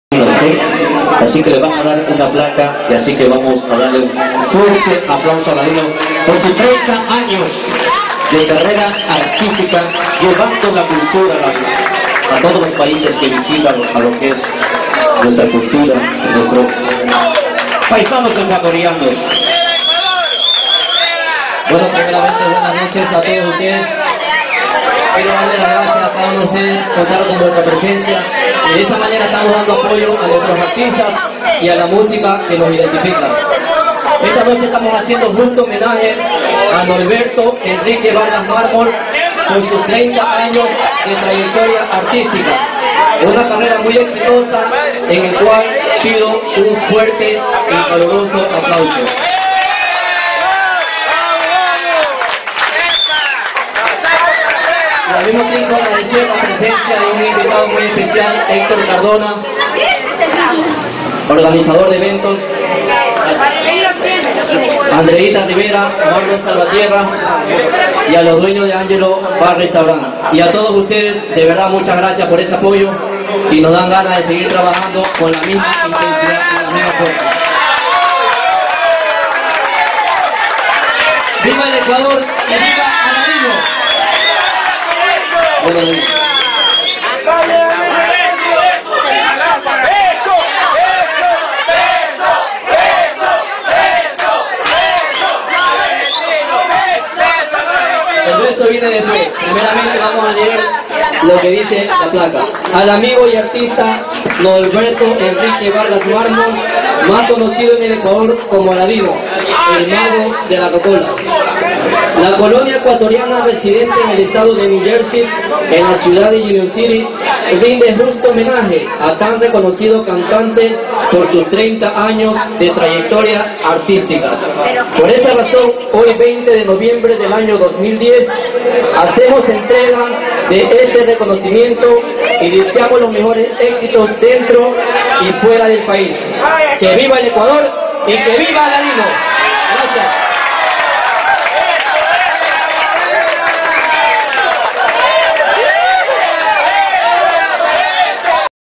ALADINO EN UNION CITY JAVIER GRANOBLE ORGANIZADOR
AQUI EL PRODUCTOR JAVIER GRANOBLE LE ENTREGA UN RECONOCIMIENTO ALADINO POR SUS MAS DE 30 AÑOS DE CARRERA ARTISTICA... MUCHAS FELICIDADES A TAN PRESTIGIOSO ARTISTA Y QUE NOS BRINDE MUCHAS MAS MUSICA..